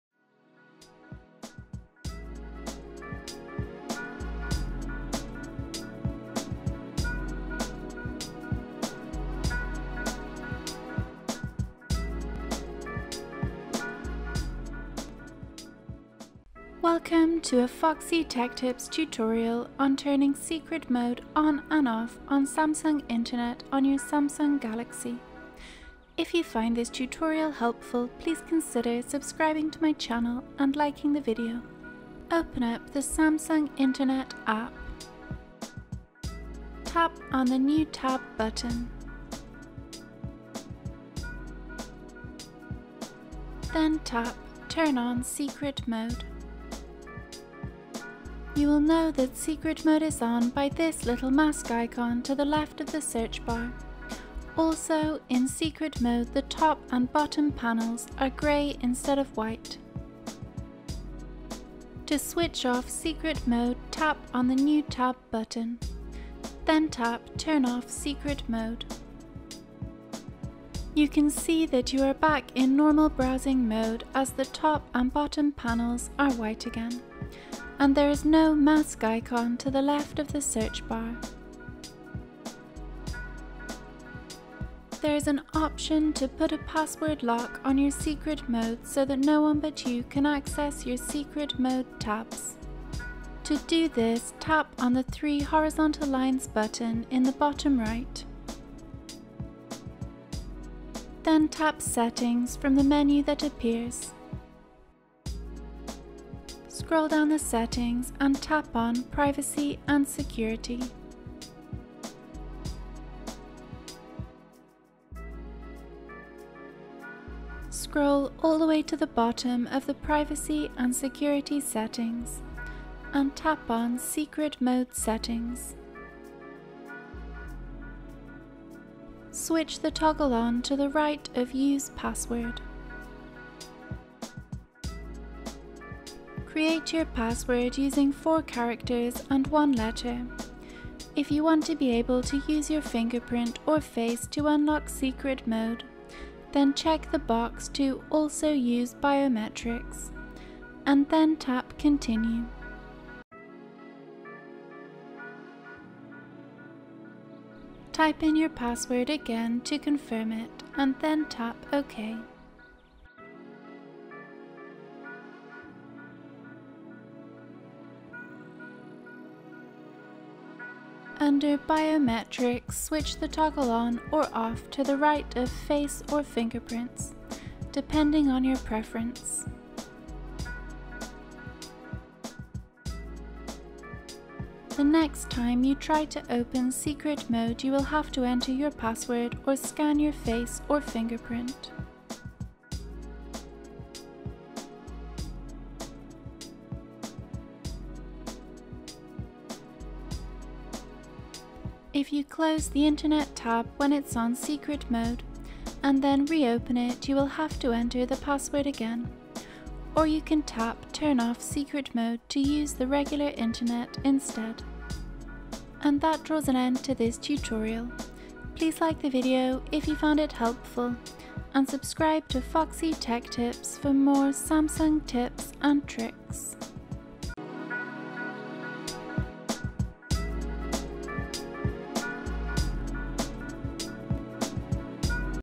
How To Use Secret Mode On Samsung Internet
Easy to follow tutorial on using the ‘secret mode’ feature on the internet app for Samsung Galaxy devices. Secret mode is similar to incognito mode on chrome , it won’t save any of your browsing history when on a secret mode tab. I’ll show you how to open and close secret mode tabs and also how to set a passcode so that you have to enter the code to access your secret mode tabs. Learn how to use secret mode on Samsung Internet now !

Steps:

Open up the Samsung internet app.

Tap on the new tab button.

Then tap ‘turn on secret mode’

You will know that secret mode is on by this little mask icon to the left of the search bar, also in secret mode the top and bottom panels are grey instead of white.

To switch off secret mode tap on the new tab button, and then tap ‘turn off secret mode’.

You can see that you are back in normal browsing mode as the top and bottom panels are white again and there is no mask icon to the left of the search bar.

There is an option to put a password lock on your secret mode so that no one but you can access your secret mode tabs.

To do this tap on the three horizontal lines button in the bottom right.

Then tap ‘settings’ from the menu that appears.

Scroll down the settings and tap on ‘privacy and security’.

Scroll all the way to the bottom of the privacy and security settings and tap on ‘secret mode settings’.

Switch the toggle on to the right of ‘use password’

Create your password using 4 characters and 1 letter, if you want to be able to use your fingerprint or face to unlock secret mode then check the box to ‘also use biometrics’ and then tap continue.

Type in your password again to confirm it and then tap ok.

Under biometric switch the toggles on or off to the right of face or fingerprints depending on your preference.

The next time you try to open secret mode you will have to enter your password or scan your face or fingerprint.

If you close the internet tab when it is on secret mode and then reopen it you will have to enter the password again, or you can tap ‘turn off secret mode’ to use the regular internet instead.